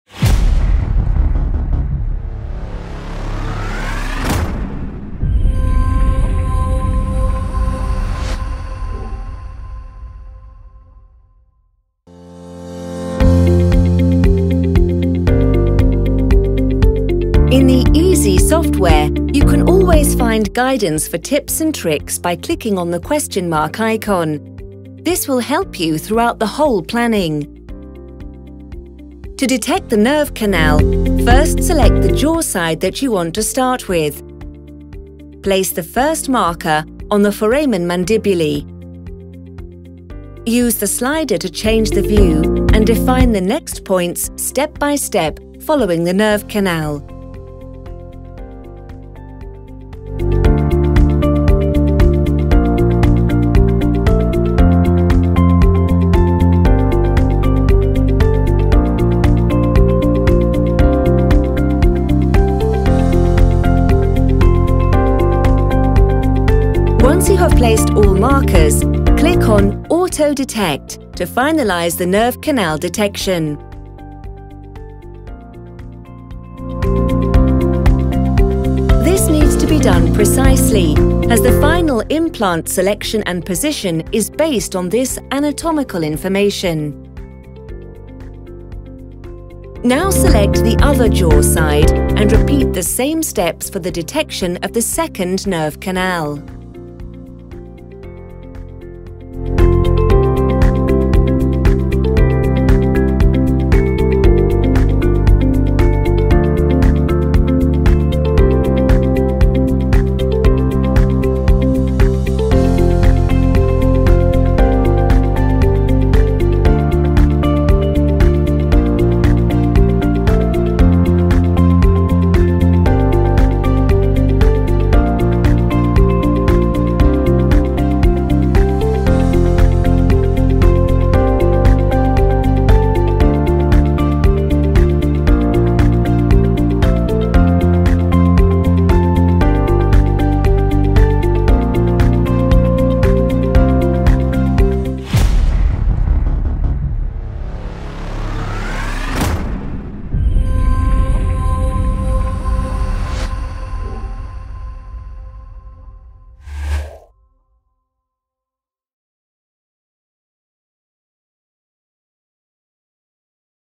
How to detect the nerve canal in coDiagnostiX® Easy Mode
Learn how to detect the nerve canal in coDiagnostiX® (Easy Mode).